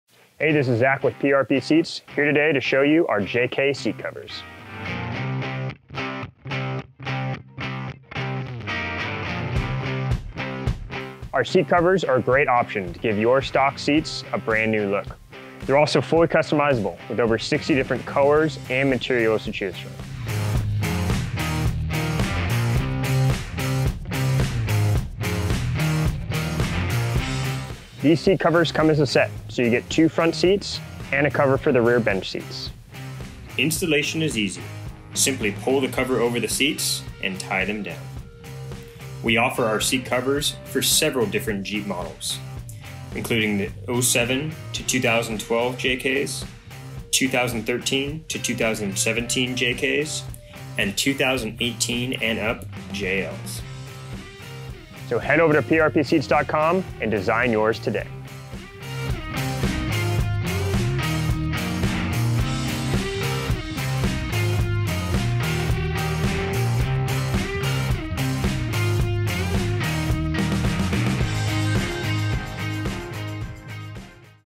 PRP Jeep Seat Covers
Introducing PRP's newest line of seat covers for Jeep Wrangler JKs and JLs.

These seat covers are easy to install, with just a few tools required. Simply slip the covers and secure them with the velcro and you'll be set to hit the road. You'll be sure to turn heads with this stylish update for your Jeep.

Everything except the tools required are supplied with the install kit.